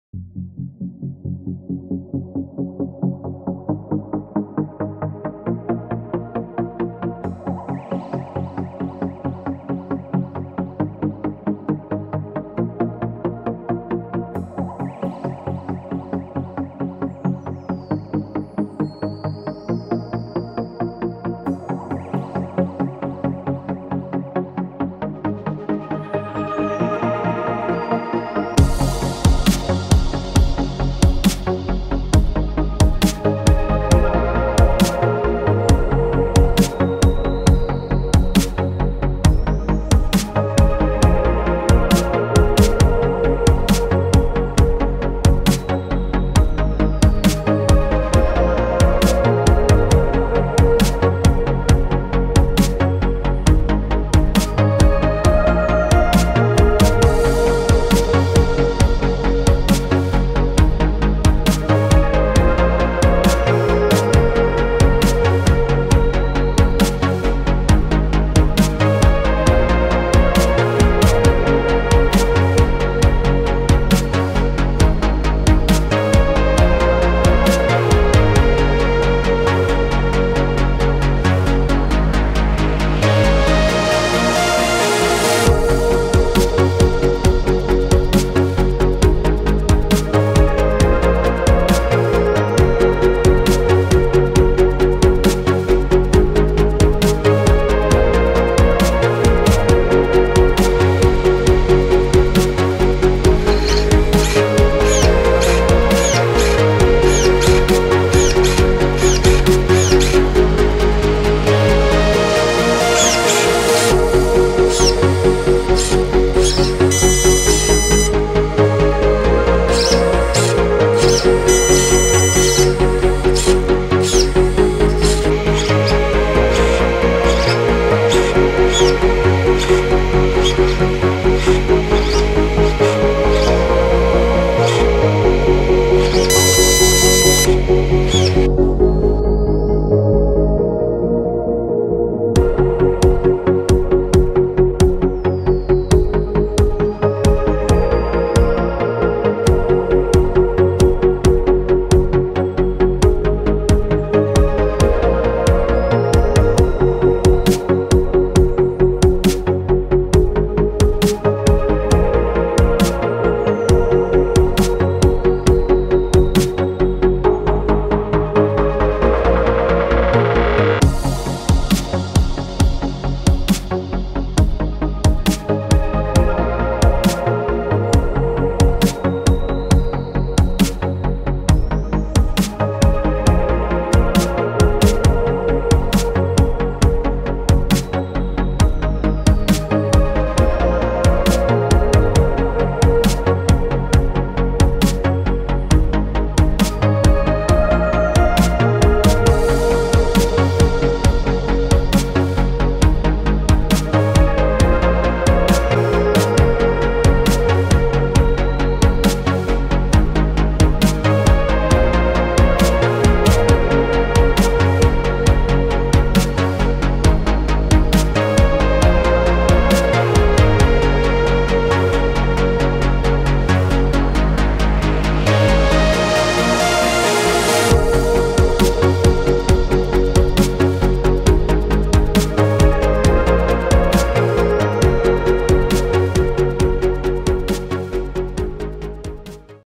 FG Sportsline 1/5 4WD Brushless Update - Upgrades
It's been awhile since I played with the on road stuff.
Quadcopters are taking all the free time I have :-)
Did a few updates including changing out the XL2 with a MGM Compro.
Hydraulic brakes are so much better than the cable disc brakes. Now I can be confident about stopping this sucker. :-)
Planing to do some runs soon.